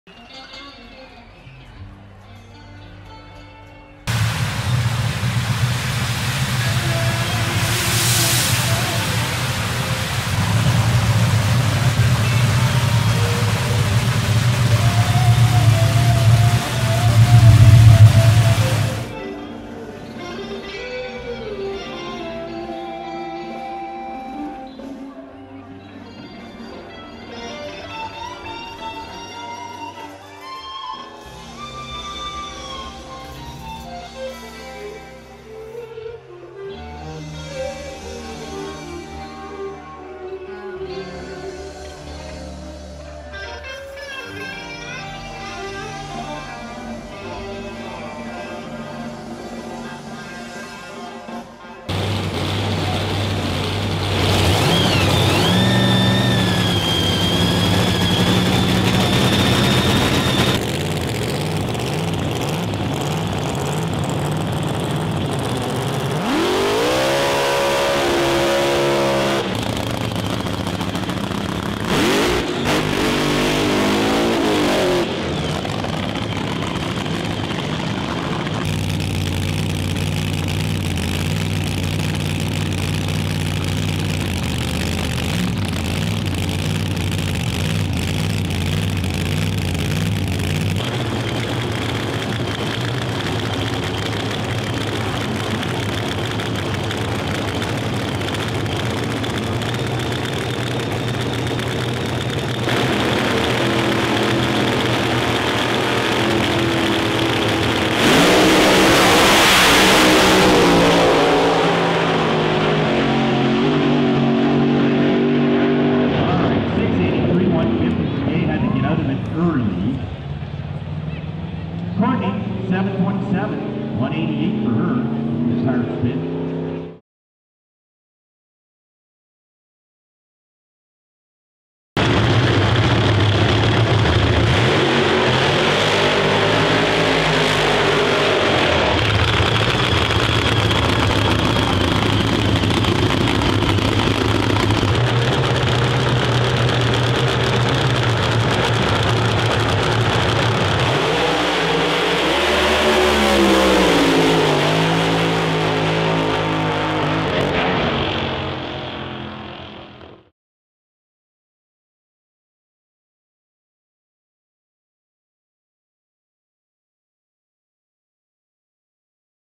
Courtney Mageau VS Jay Mageau Hot August Night 2018
Courtney Mageau BB/FC
VS
Jay Mageau AA/FC
Hot August Night 2018
Castrol Raceway Edmonton, AB